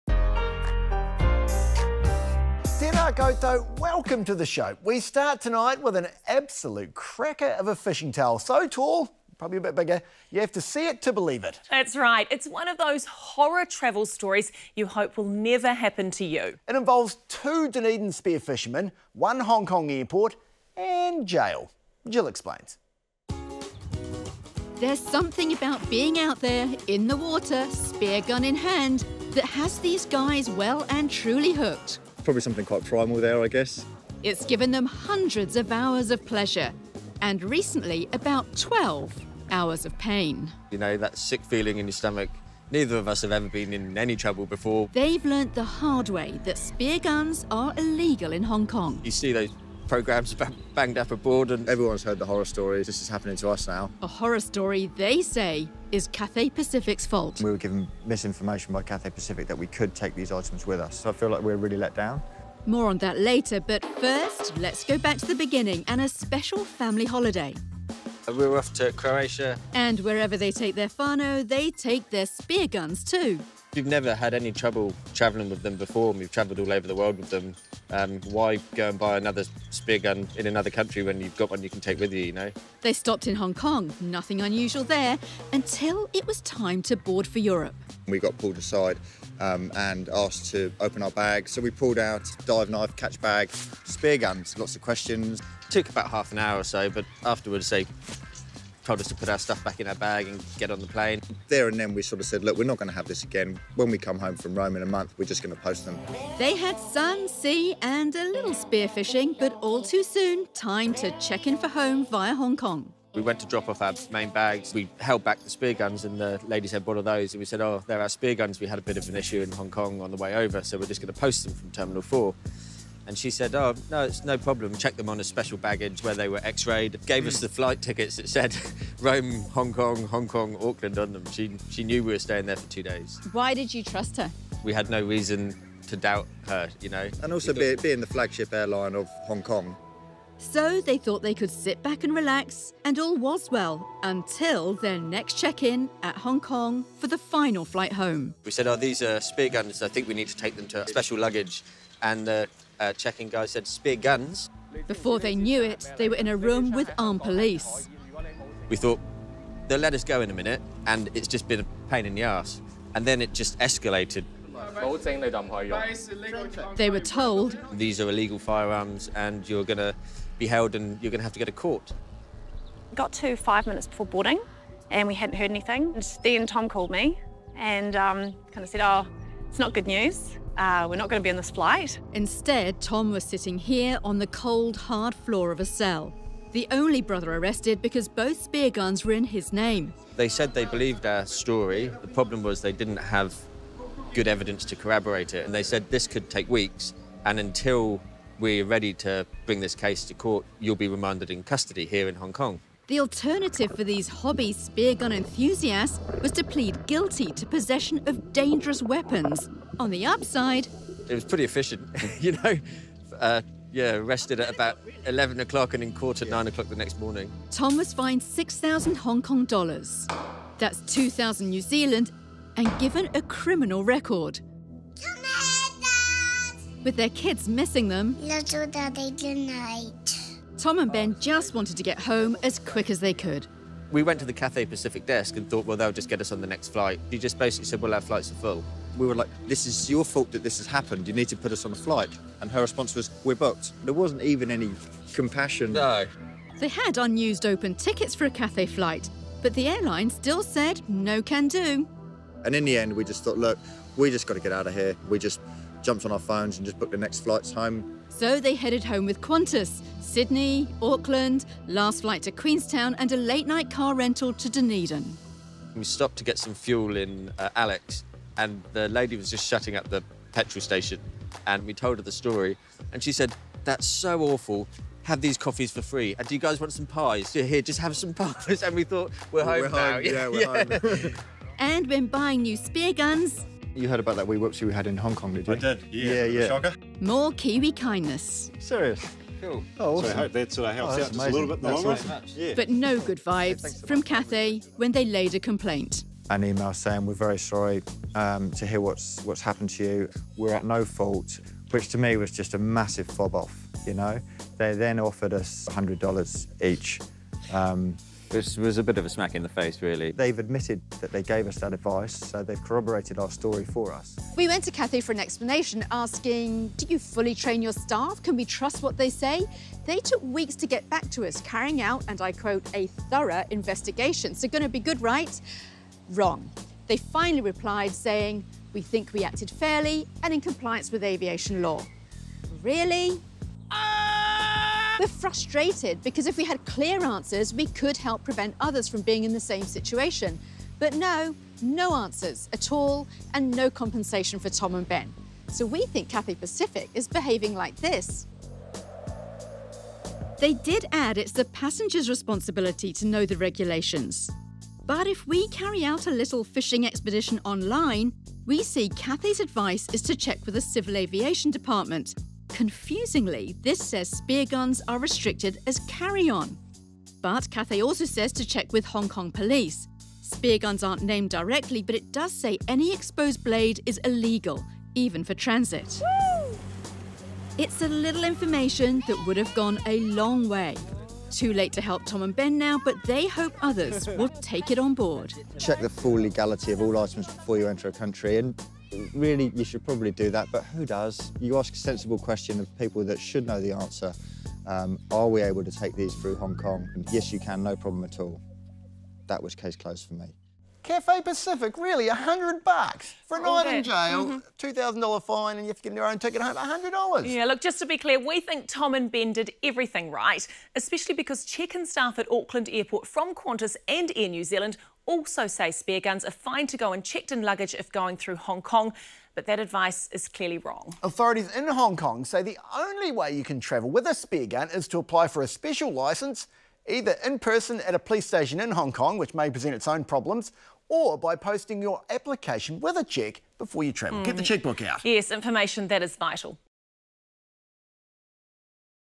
Handcuffed in a foreign country: Fishing trip turns horror story for Dunedin pair
The men found out the hard way that spearguns are illegal in Hong Kong. They blame their airline for bad advice.